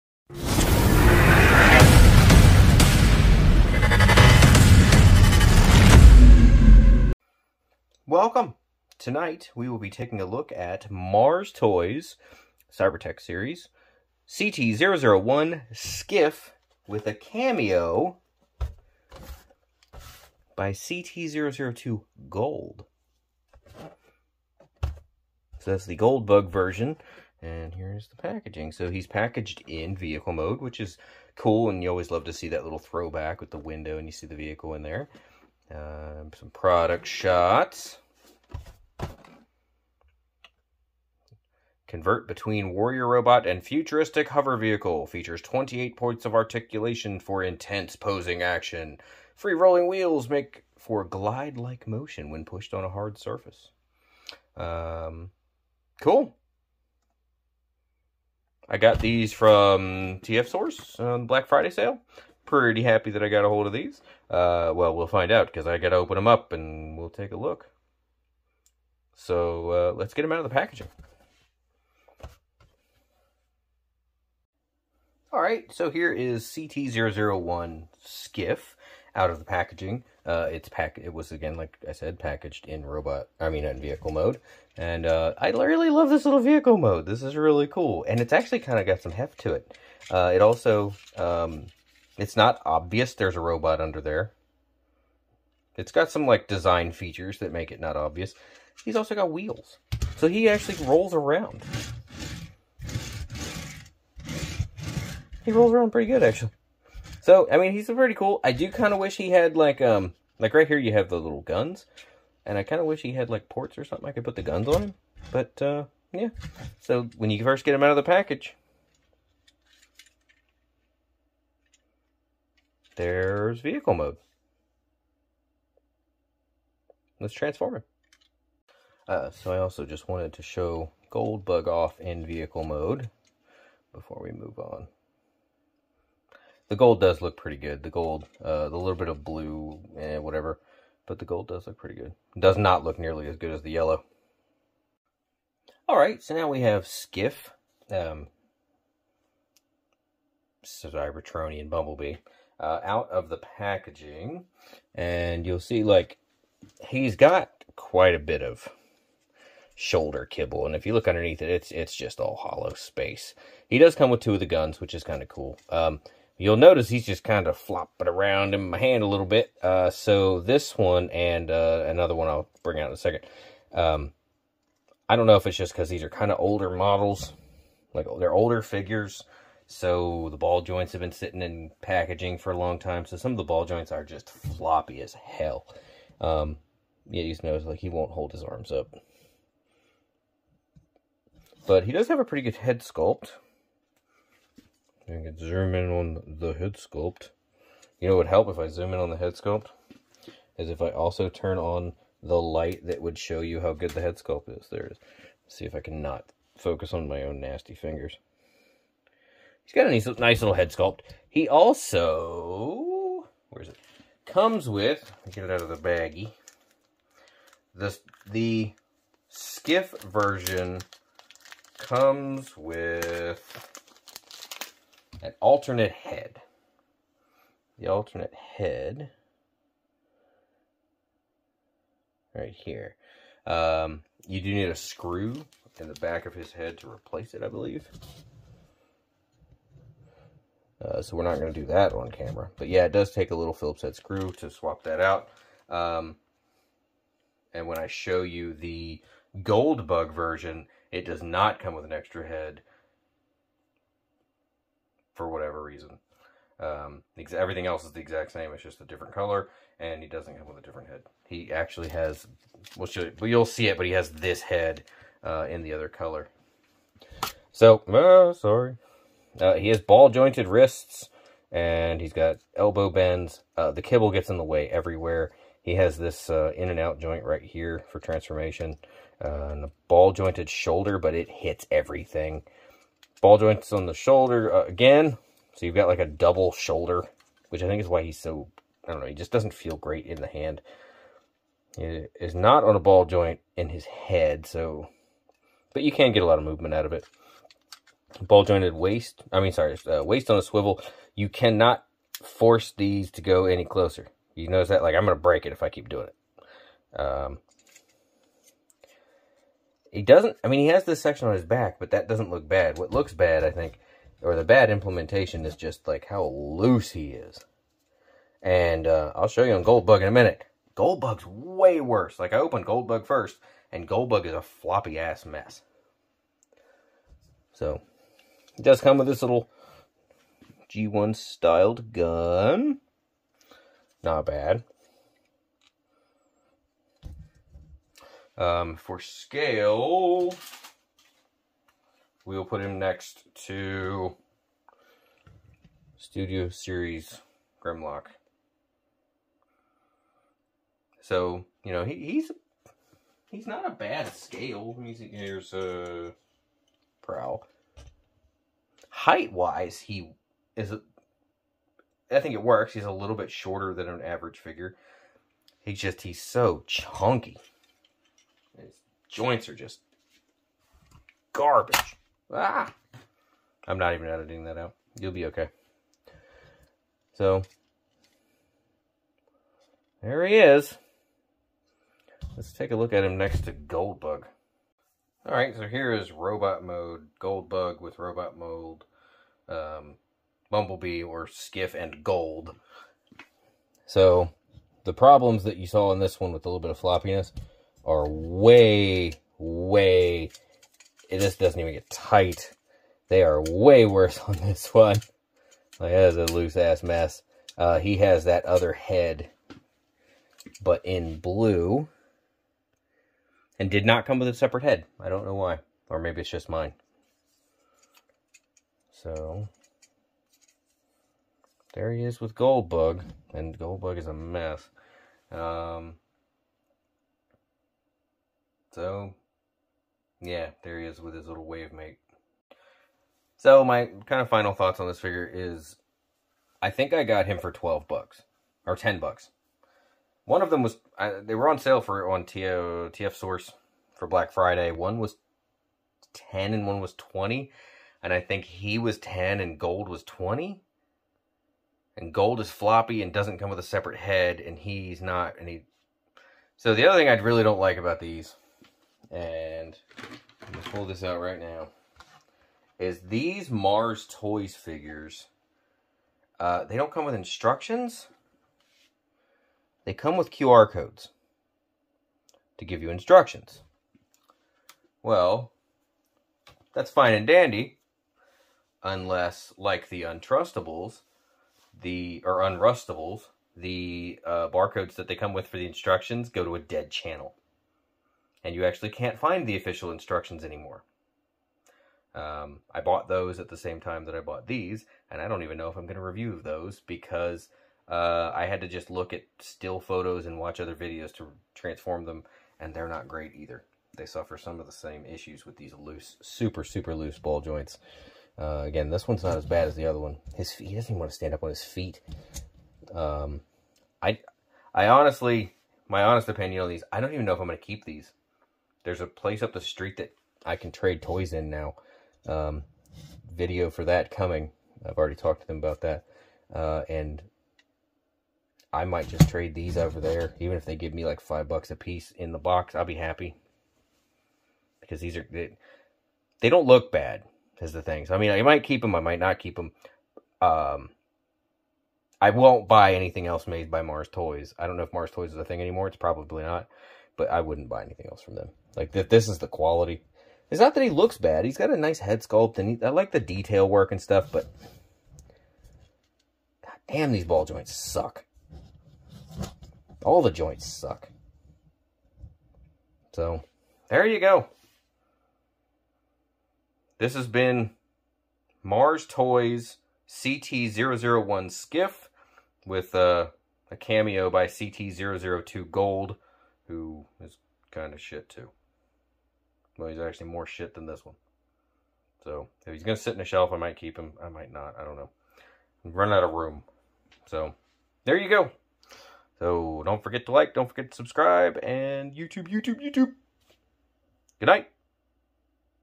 Firetox Reviews - Maas Toys - CT001 Skiff (Cybertronian Bumblebee)
The closest thing we had to a G1 episode 1 Bumblebee till the official one came out

NEW ETSY STORE!

MERCH!